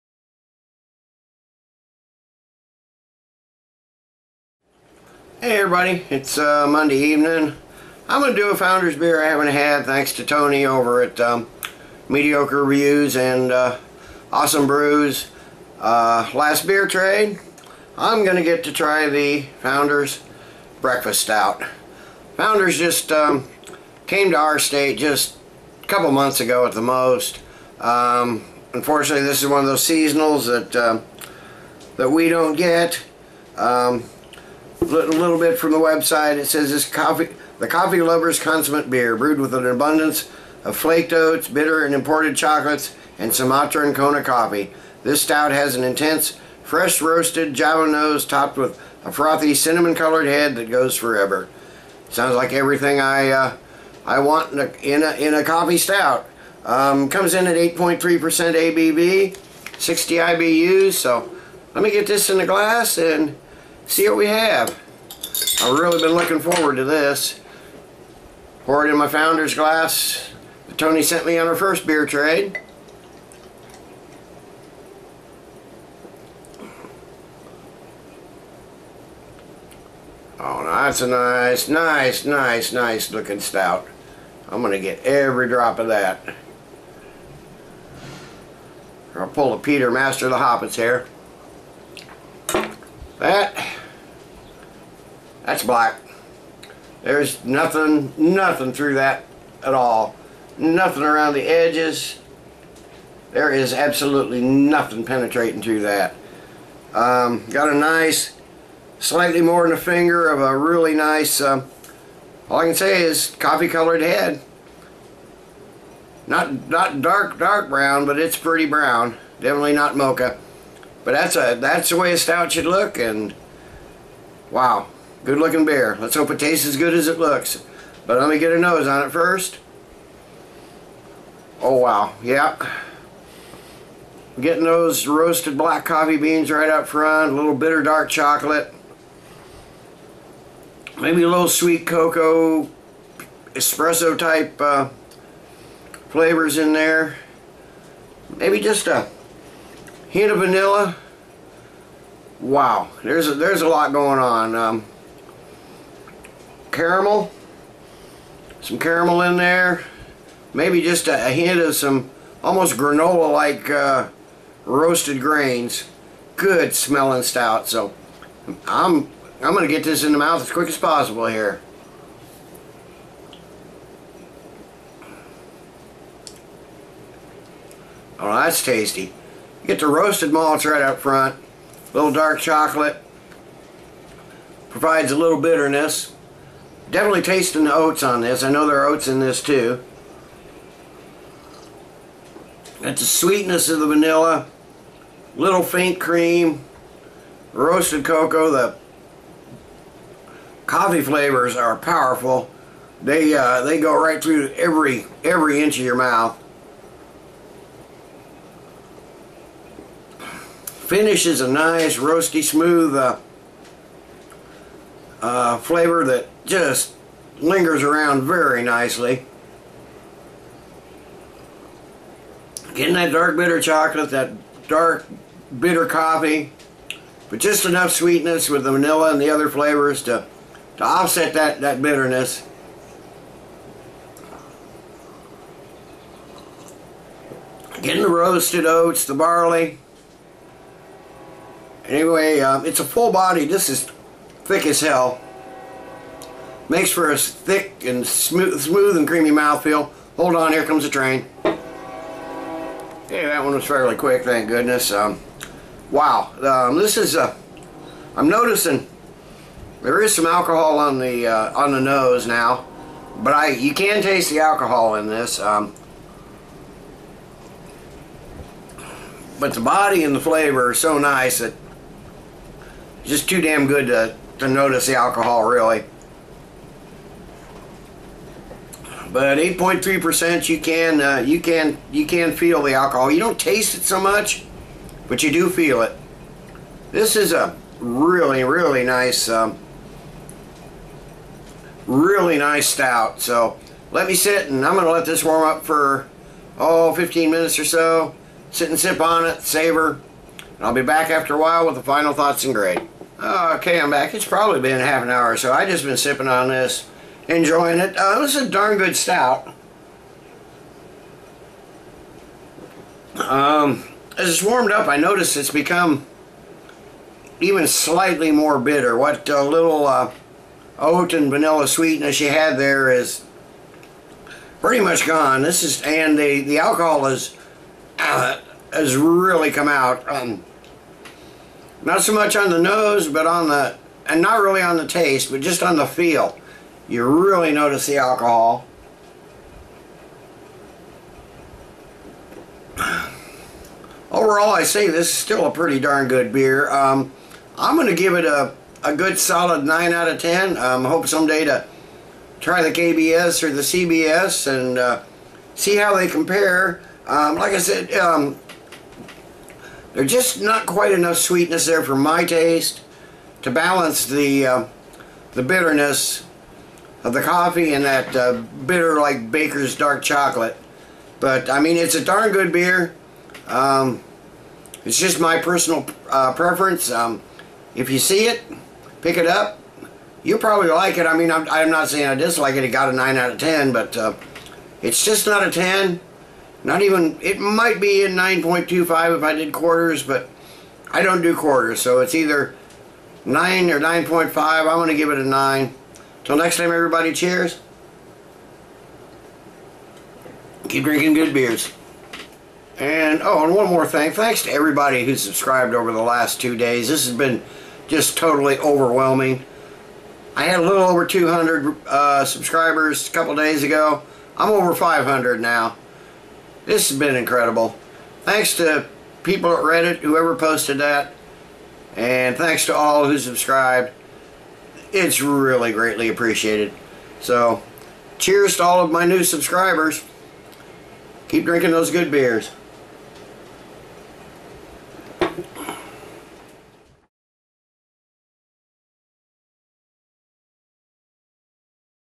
Founders Breakfast Stout Keith's Beer Reviews # 330 (and 500 subscribers thank you)
Founders Breakfast Stout

The coffee lover's consummate beer. Brewed with an abundance of flaked oats, bitter and imported chocolates, and Sumatra and Kona coffee, this stout has an intense fresh-roasted java nose topped with a frothy, cinnamon-colored head that goes forever

You've got to love coffee to truly appreciate this phenomenal brew. Brewed with an abundance of flaked oats, bitter and sweetened imported chocolates, Sumatra and Kona coffee. We're actually not sure if this is some type of coffee cake or a beer. Either way you can drink this ale with a fork. Breakfast Stout has an intense fresh roasted coffee nose toped with a cinnamon colored frothy head that seems to never fade and makes you wish breakfast could last forever.

8.3% ABV
60 IBUs